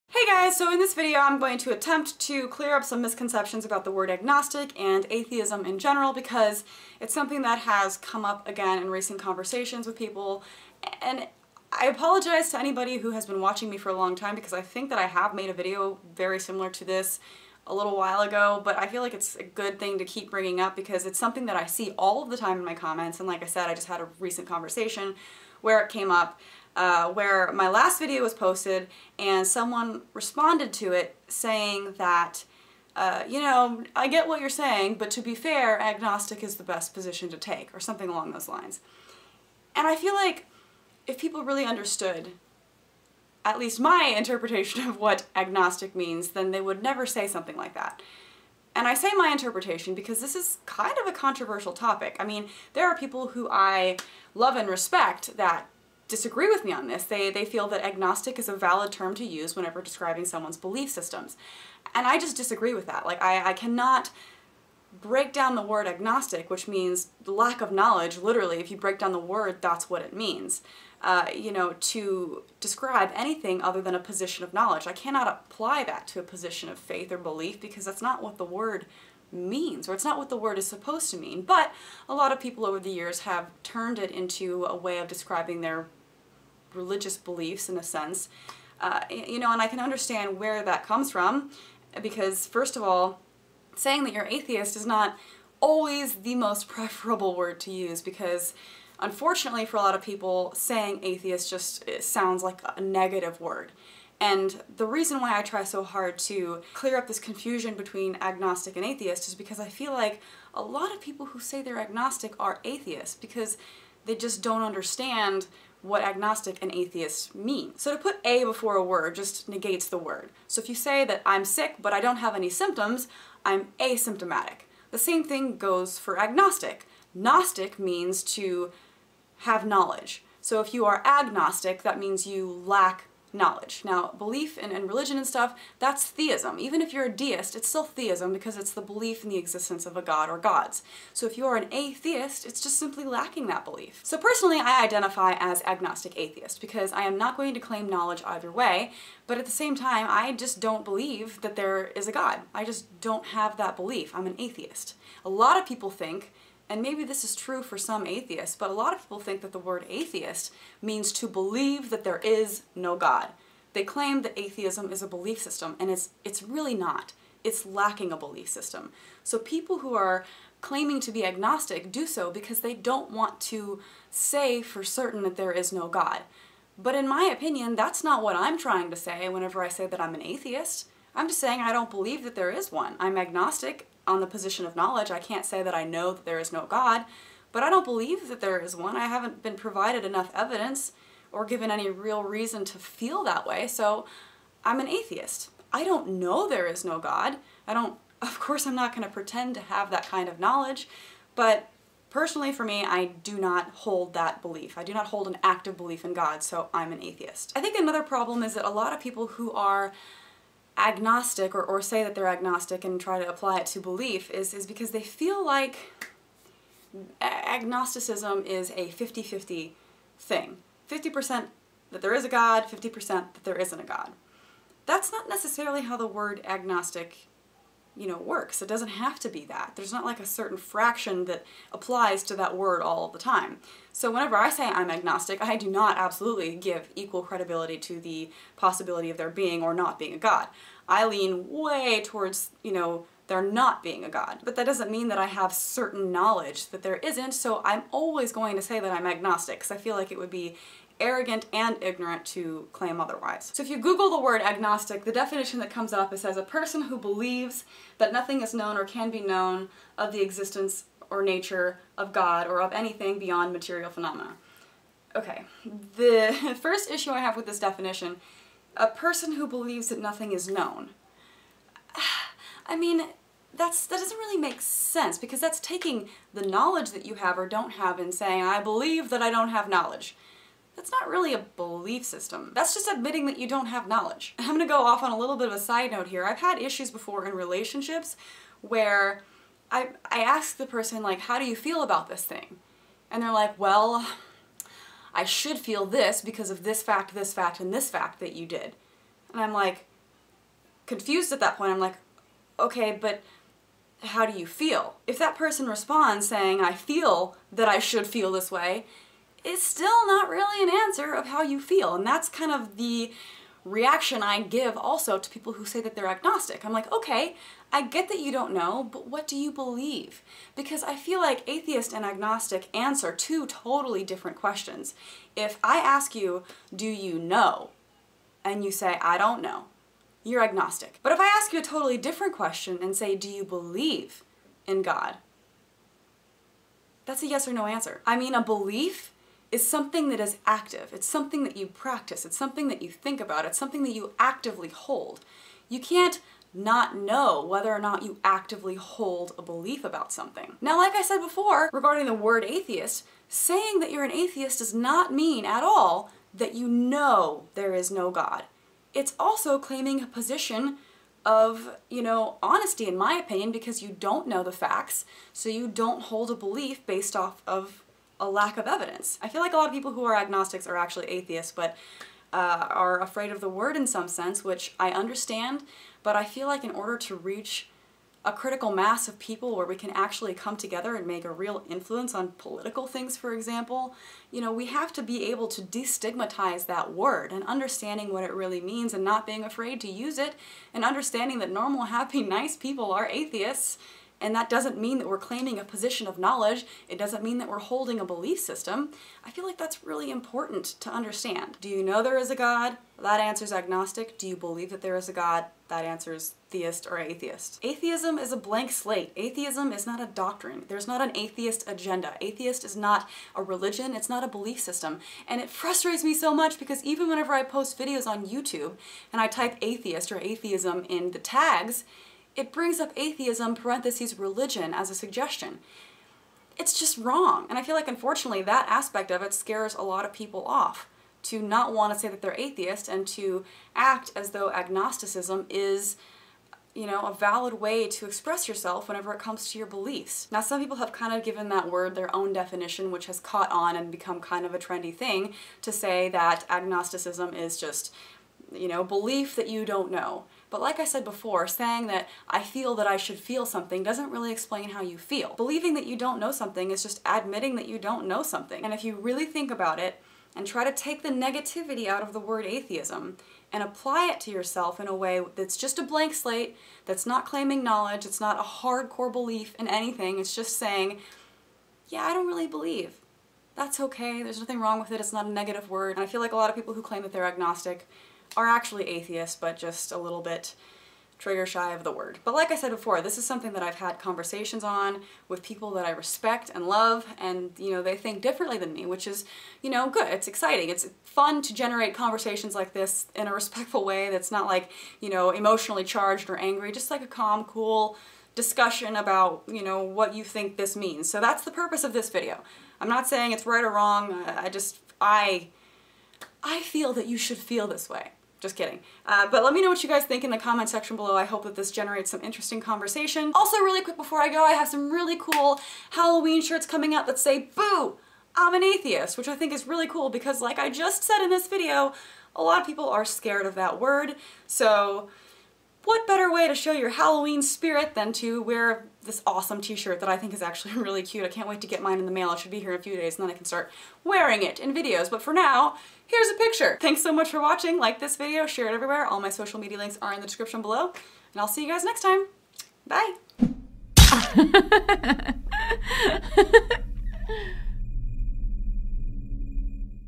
ATHEIST VS AGNOSTIC
Discussing the meaning of Atheist and Agnostic. If you don't believe in god, you're an atheist... even if you're not certain god doesn't exist.
"BOO! I'm an ATHEIST" Tshirt

I only accept friends that I know, but this is if you want to follow my personal facebook

Do you identify as just agnostic? Are you afraid that labeling yourself as an atheist will bring negative attention towards you? What about saying agnostic atheist? I've called myself agnostic atheist for a while now and it personally makes the most sense to me. Agnosticism is not a religion (and neither is Atheism), despite the youtube tag suggestions haha. ATHEIST VS AGNOSTIC! GO!